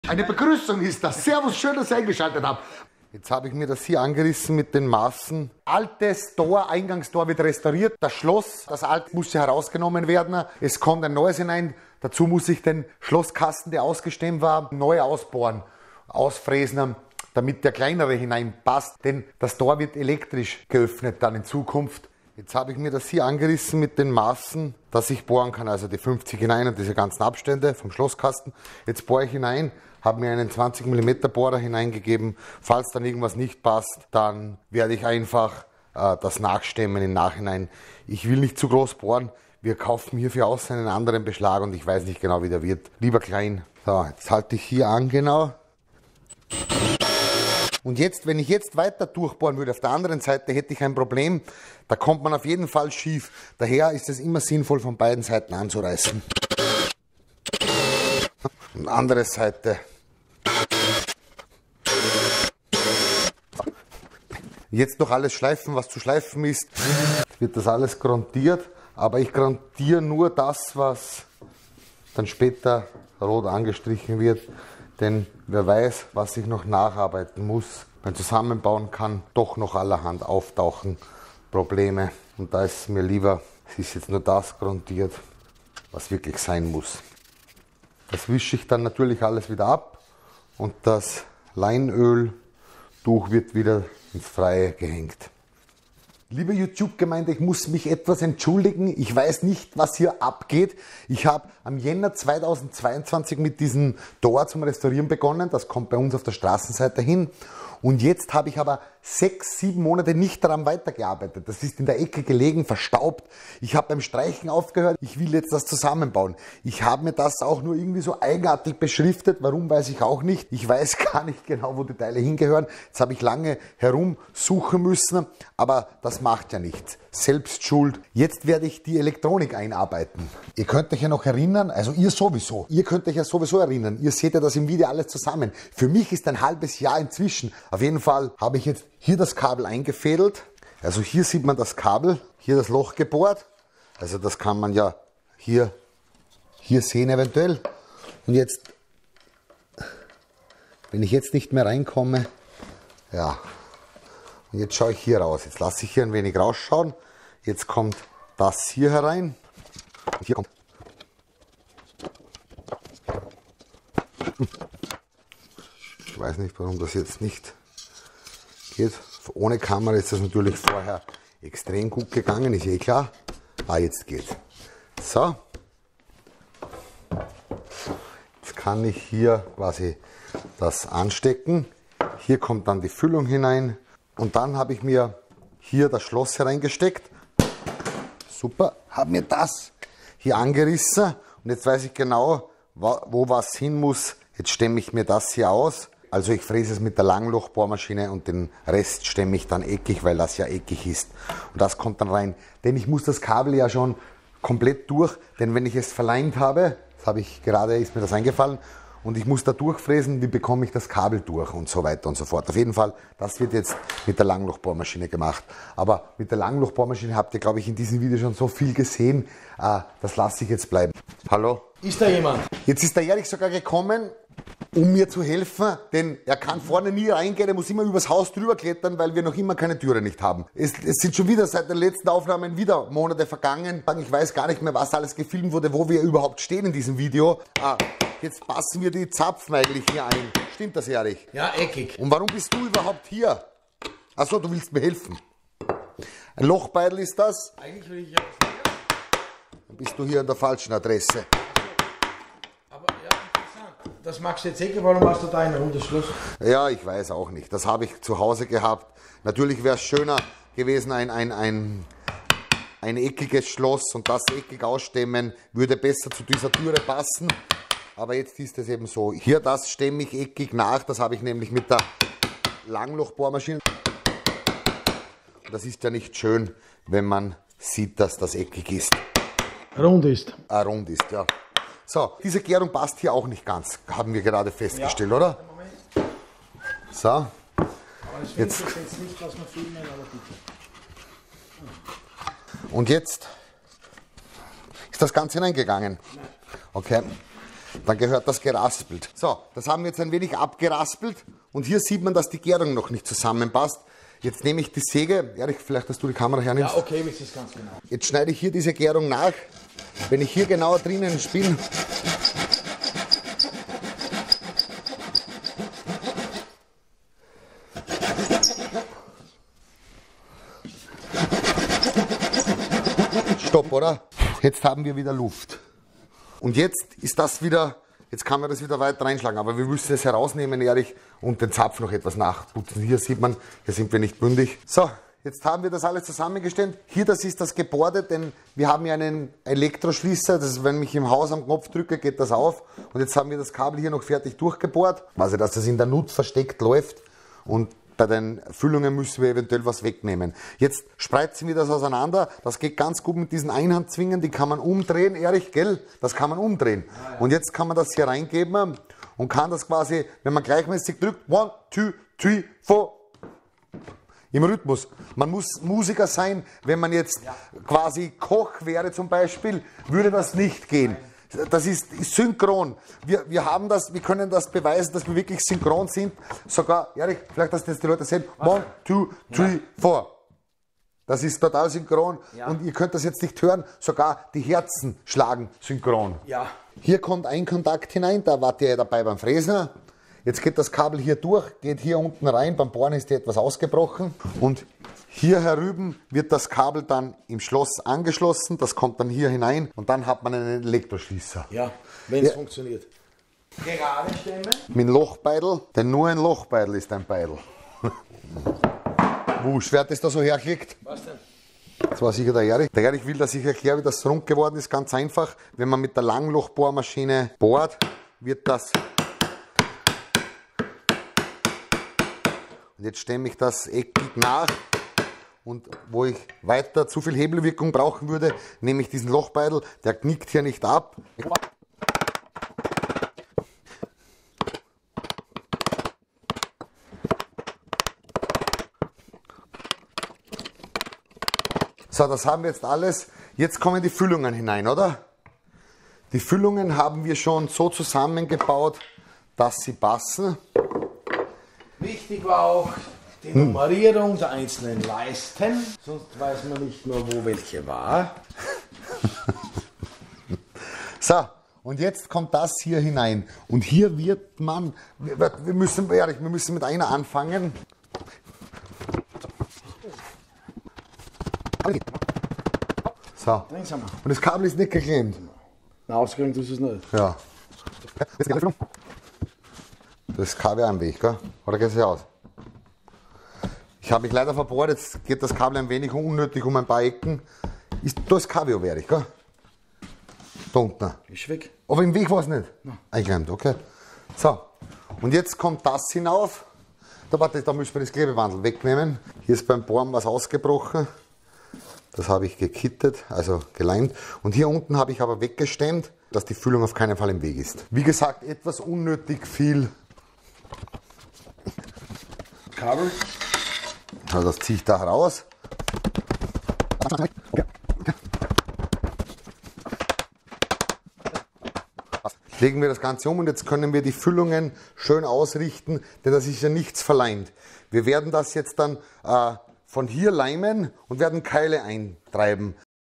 Tür zusammensetzen mit Knochenleim - Eingangstor von 1890 restaurieren Teil 13
Feinarbeit für den Schlosskasten und Rahmen und Füllungen zusammenbauen. Manuel verwendet dazu die alte Knochenleimtechnik, die er sich am Werkstück abgeschaut hat.
15 Euro Seestörung:
Unschlagerbar!:
3 x 3 Zwölf:
Kamera: Manuel Hafner
Schnitt: Nadja Milfait